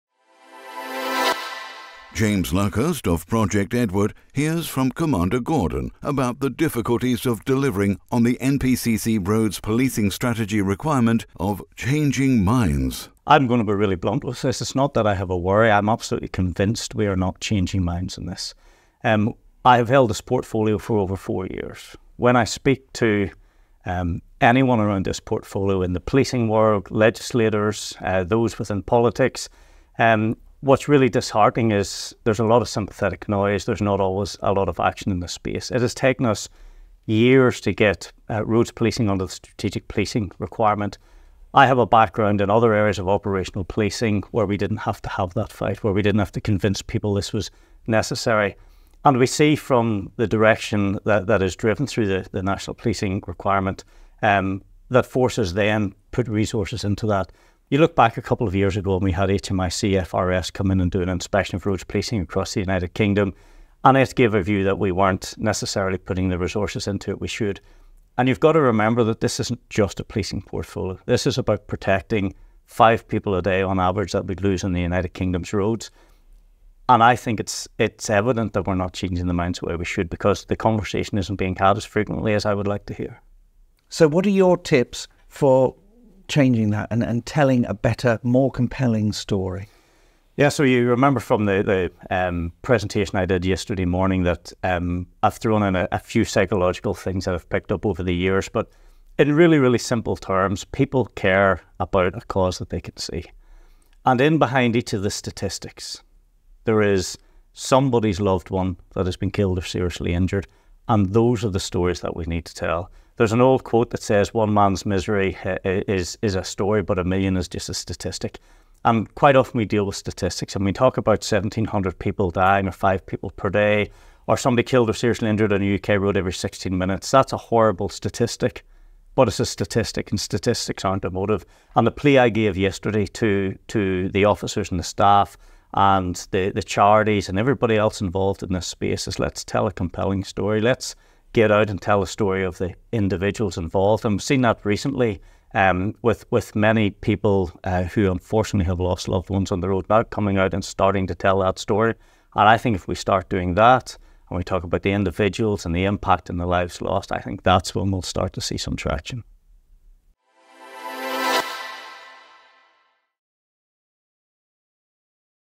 Drug 24 1 Kyle Gordon 720P
Project Edward hears from Commander Gordon, Metropolitan Police, about the difficulties of delivering on the requirement – central to the current NPCC roads policing strategy – of ‘changing minds’ and improving driver behaviour.